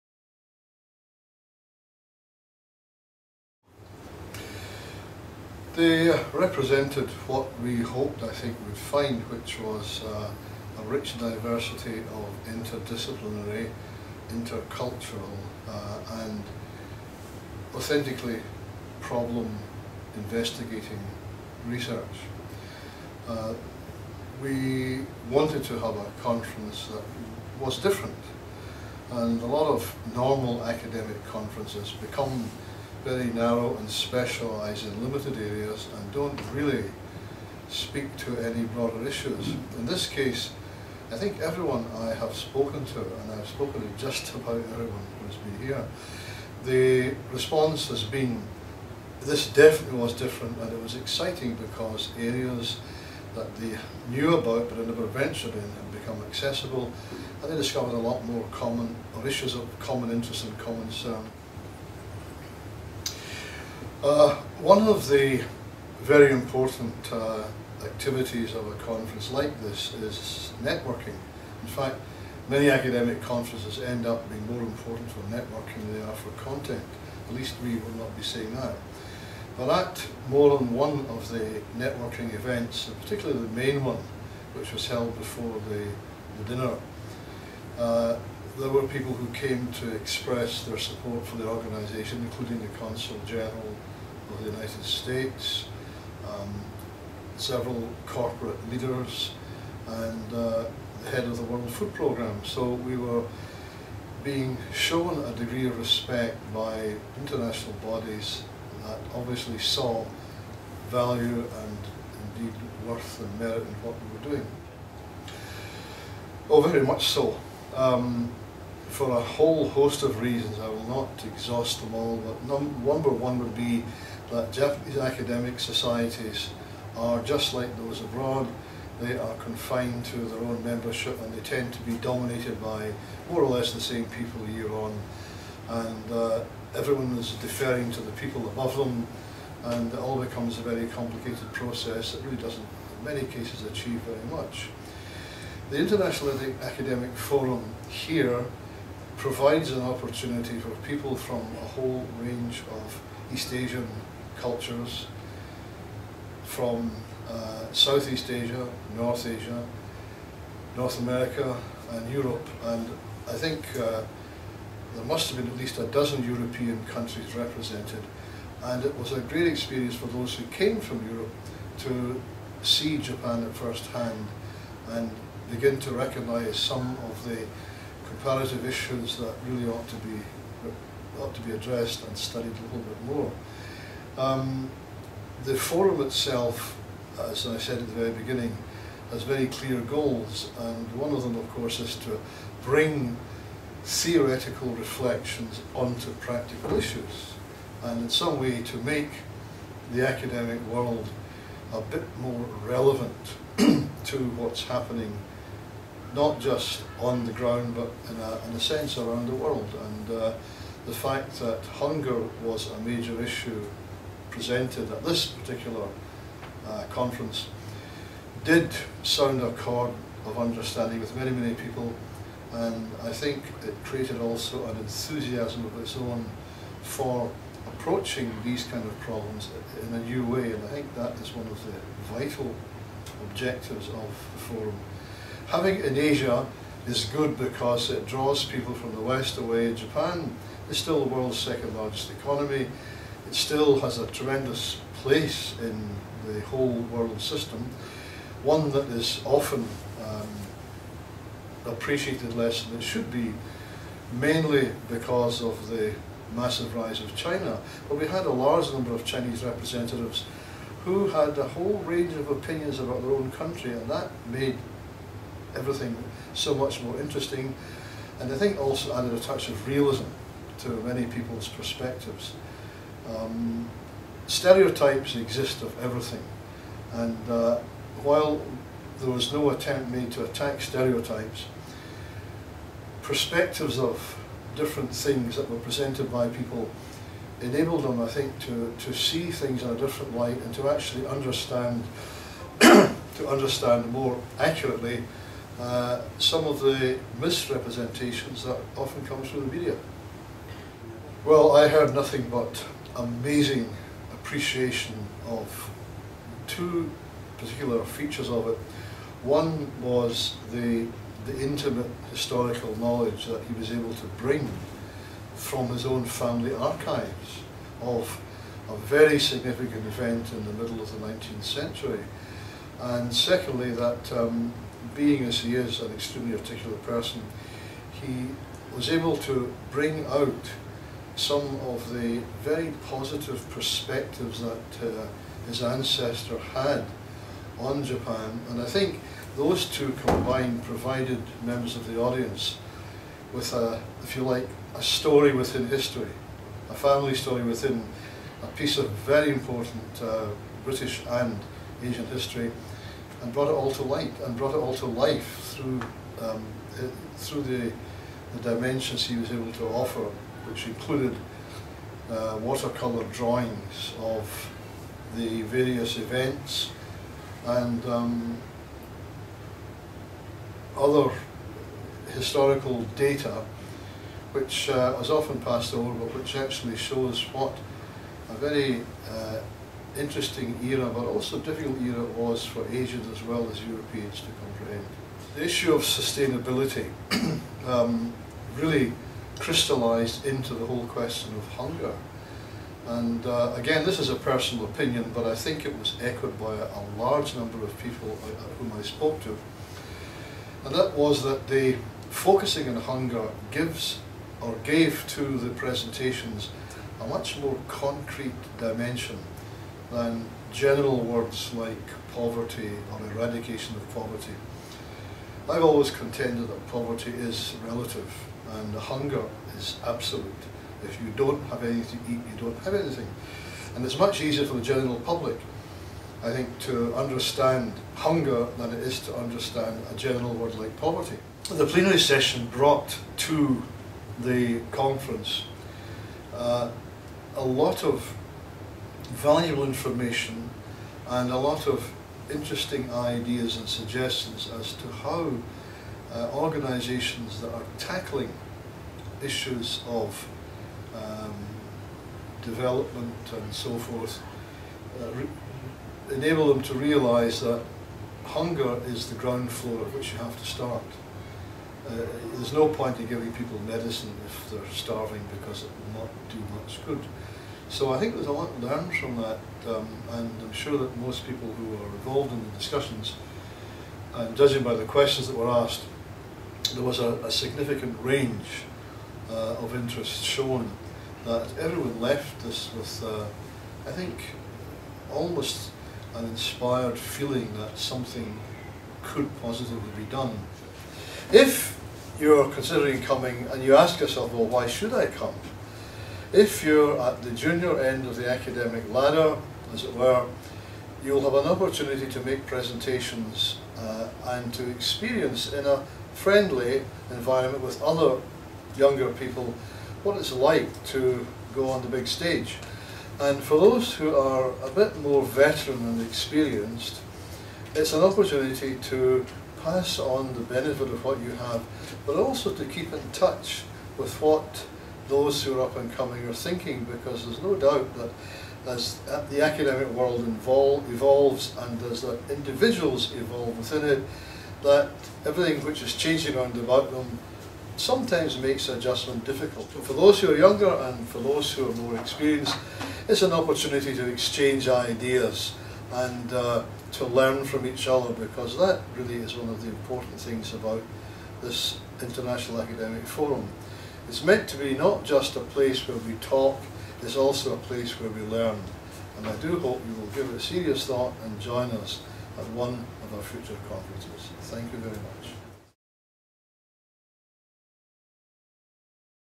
Interview with Professor Stuart D. B. Picken, Chairman of the IAFOR International Advisory Board
An interview with Professor Stuart D. B. Picken, Chairman of the IAFOR International Advisory Board